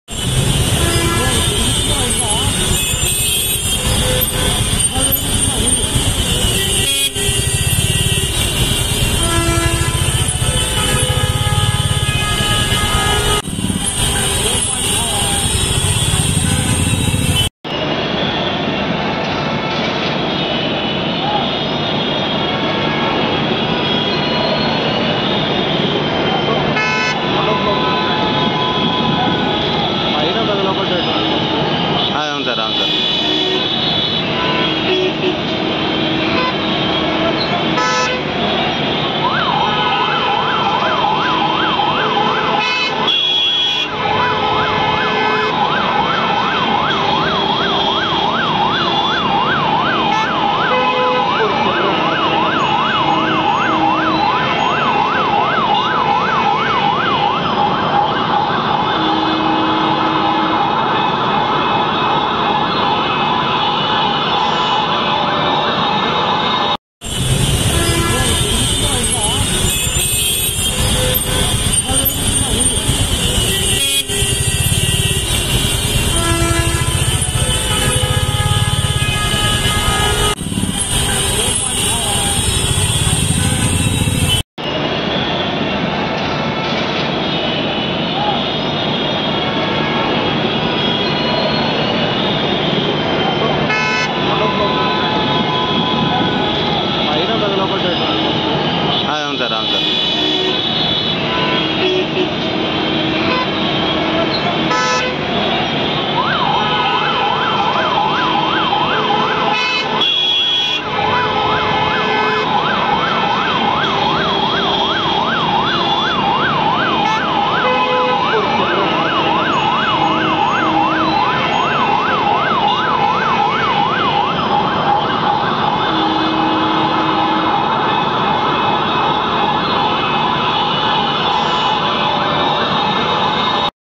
Hyderabad News : Huge fire accident in Ankura Hospital | గుడి మల్కాపూర్
breaking news : major fire accident at ankura hospital in hyderabad

హైదరాబాద్ గుడి మల్కాపూర్ అంకురా హాస్పిటల్ లో భారీ అగ్ని ప్రమాదం